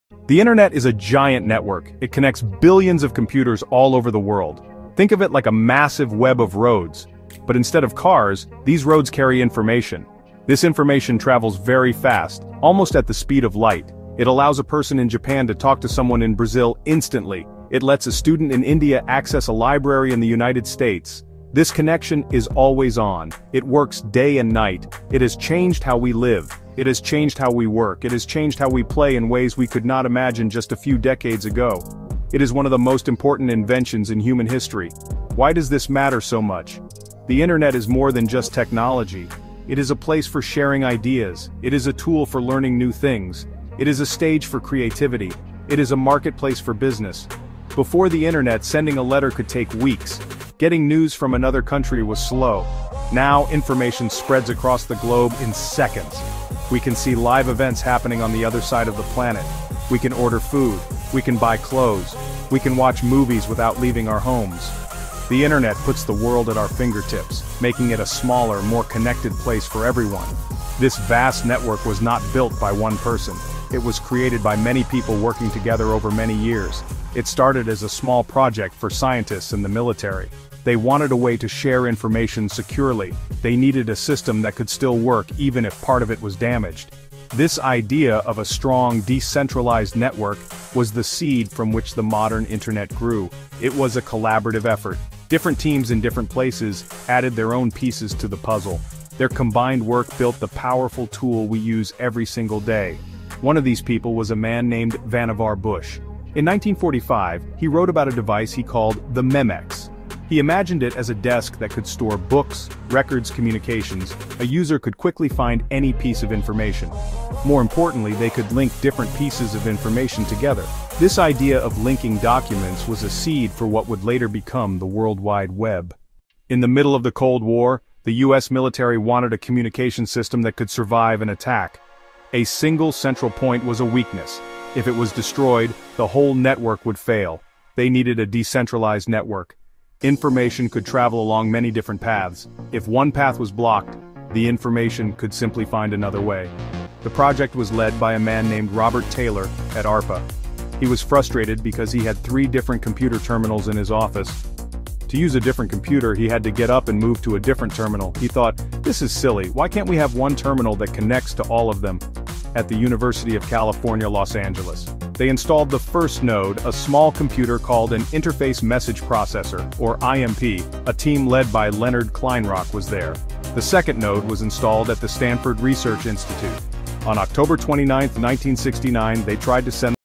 HISTORY OF THE INTERNET
Trace the Internet’s epic timeline, from ARPANET and the birth of TCP/IP to the World Wide Web, browsers, search engines, social media, and today’s TikTok-driven culture.
This educational video uses a clear English voiceover and stock footage only, guiding viewers through major milestones, key technologies, and cultural shifts that shaped the global network.
Perfect for students, tech enthusiasts, and history lovers seeking a concise timeline journey of Internet evolution.